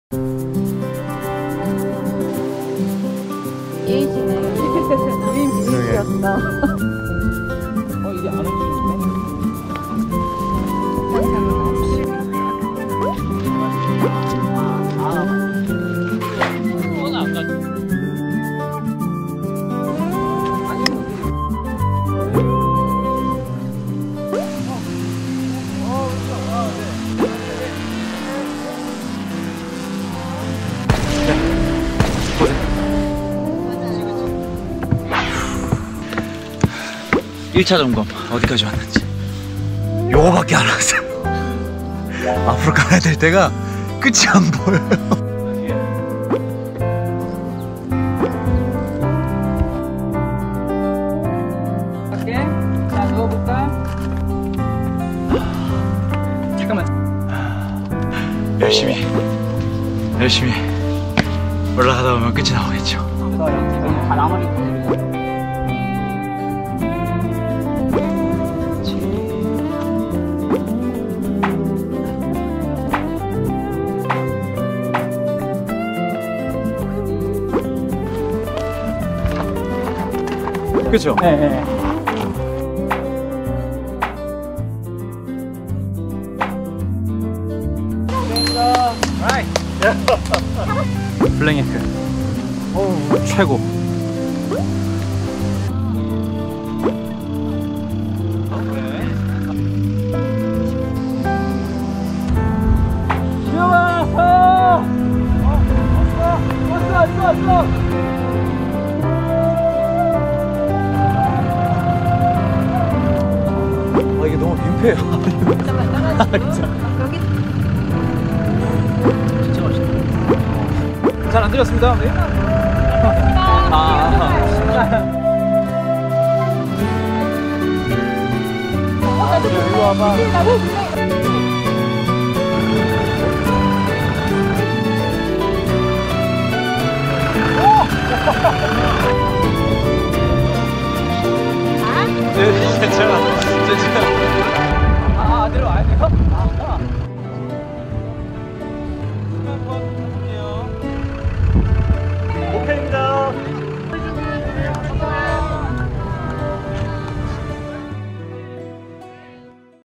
블랙야크 2020 FW BAC campaign 메이킹필름
강하늘의 '가을을 걷다' 촬영 비하인드를 영상에서 확인해보세요.